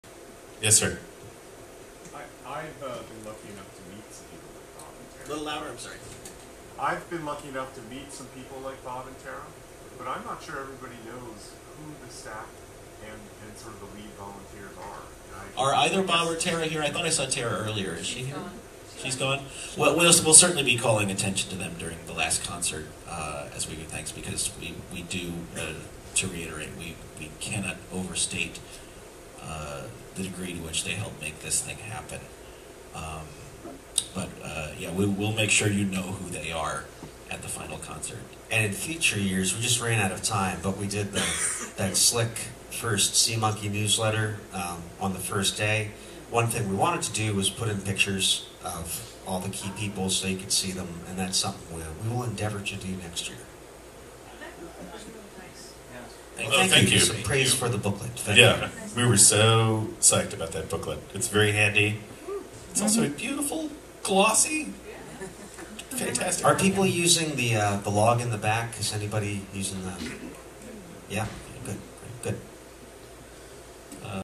Not everyone knows who the head helper monkeys are — JoCo Cruise 2015 Q&A
Someone points out that not everyone knows who all the staff or head helper monkeys (Bob and Thera) are. Apparently the Home Office ran out of time to put pictures of them all in the glossy Sea Monkey magazines we were handed out at registration, but they'll be sure to make a big deal of them at the final show.